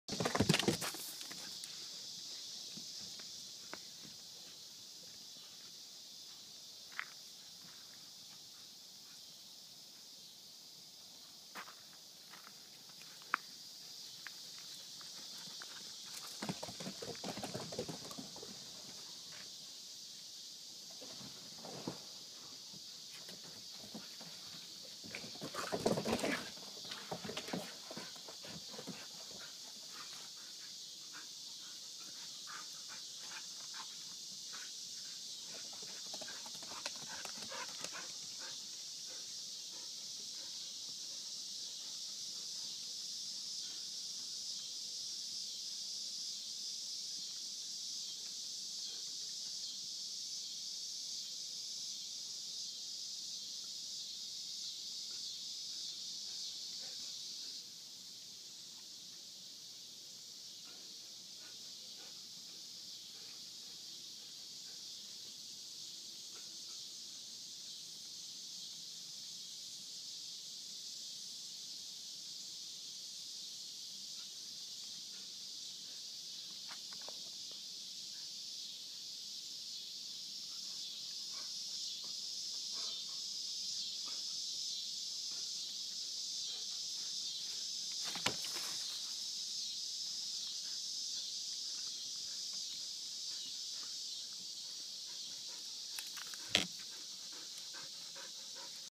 If on she could walk up on sheep this slow....
Claire the Groenendael plays with Luke the Mudi and stalks him at the end.